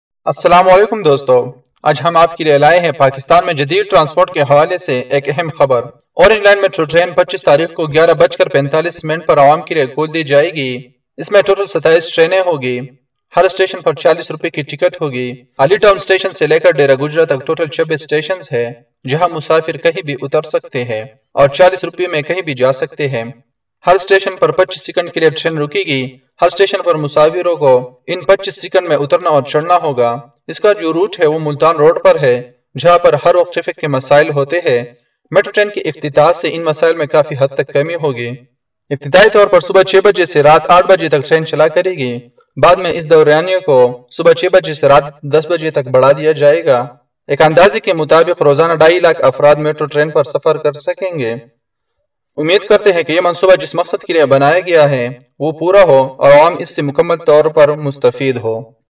Lahore Metro Train Inauguration, PTI another milestone Achieved. BRAVO PTI
Orange Line Metro Train Lahore Inauguration Ceremony To Take Place Tomorrow. A real milestone by the PTI government. It will indeed turn to be a successful project that will benefit the common citizens of the region.